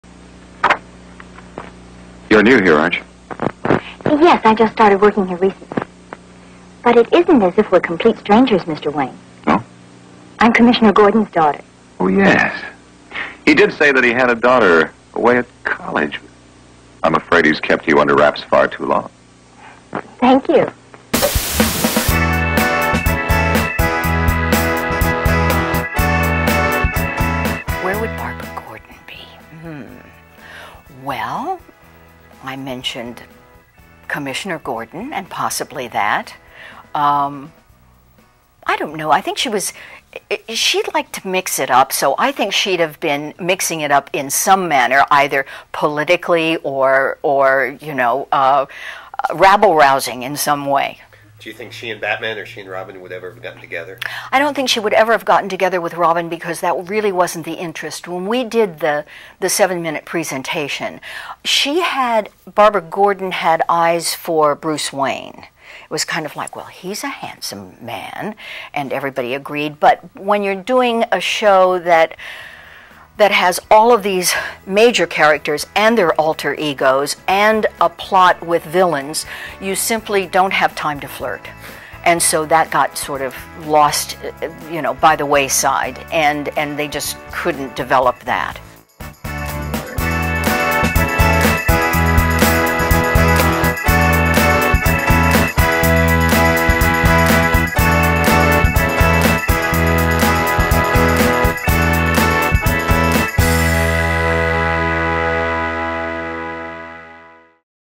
Yvonne Craig Where would Barbara Gordon-Batgirl be Today?
In this archival interview clip, the late Yvonne Craig speculates where her iconic 1966 BATMAN series character, Barbara Gordon, aka Batgirl, would have ended up.

Thanks!